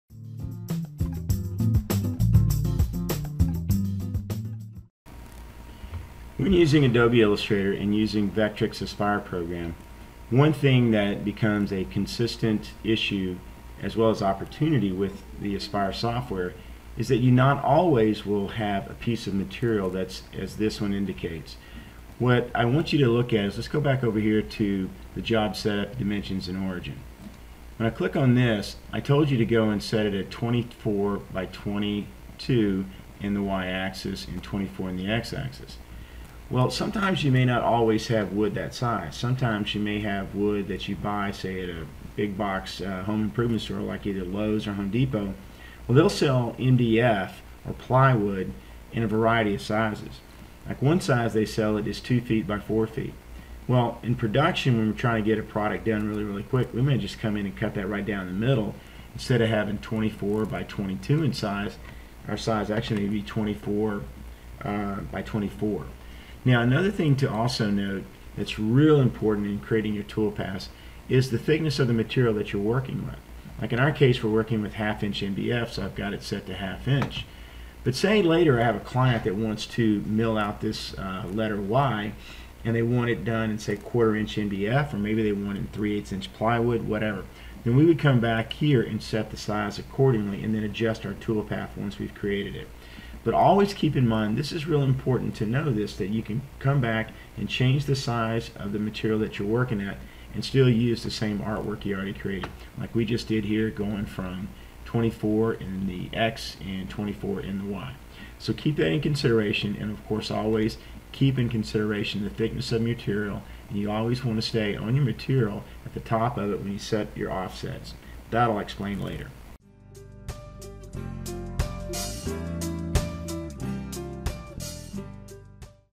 Tutorial 3 - From Adobe Illustrator to Vetric Aspire to CNC Router Letters
How to use Adobe Illustrator CC to design a letter. Then to use Vetric Aspire to create a CNC Tool Path using the Adobe Illustrator CC letter design.  Then use an Open Source CNC Router, that is controlled by Mach 3 to mill out the MDF Wooden Letter.

This is Tutorial 3.  This tutorial covers more about the materials setup and working with different sizes and thicknesses of material.  Such as, one job may require .25” MDF and another job of the same tool path requires .375” thick plywood.  This tutorial covers those contingencies how to modify the job size.